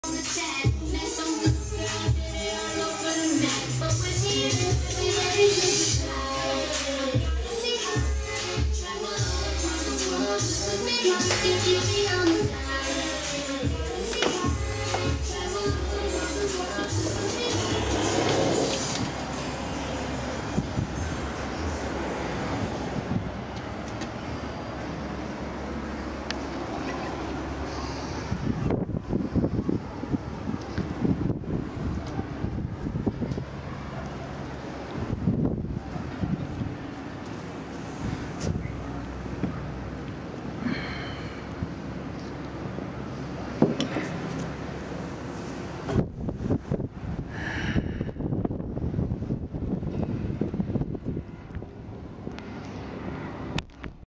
Home office lunch
Have no canteen here, so had to settle for a lunch out on the terrace